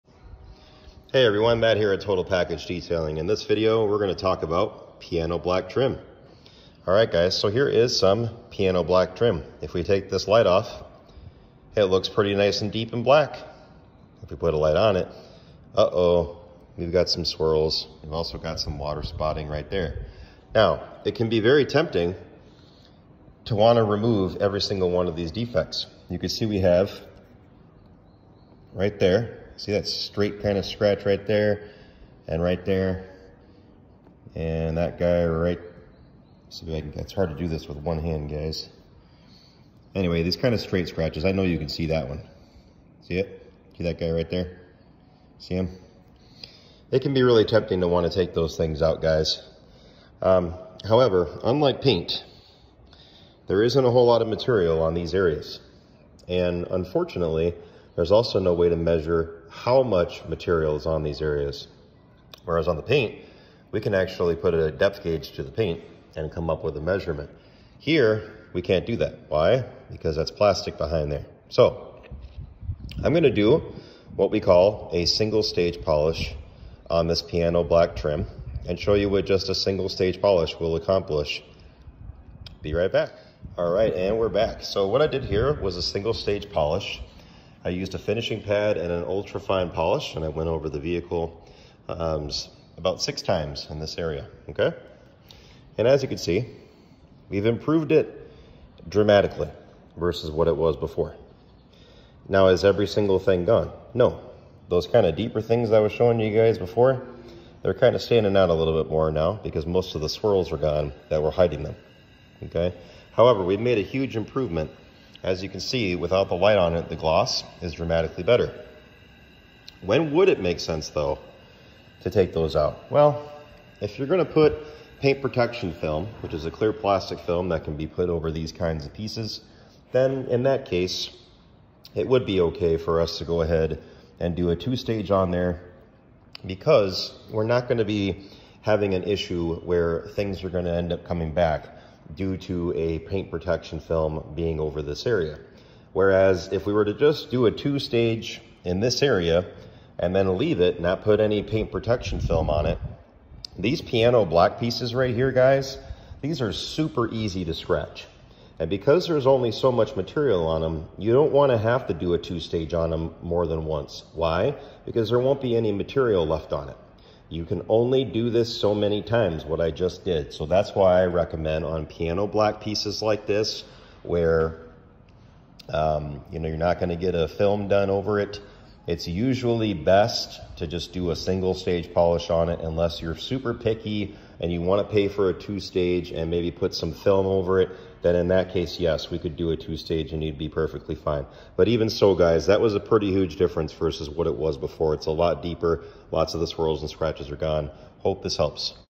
🎹 Piano Black Trim. How to best care for it.
In this video, I discuss black plastic trim, and how to best properly care for it .￼

￼￼ Black plastic trim usually does not have a lot of material on it with that said it’s best not to get too aggressive unless you’re going to put a paint protection film on it I recommend sticking to our single stage polish so as to keep as much material on there as possible. ￼￼

￼We are located in Appleton WI at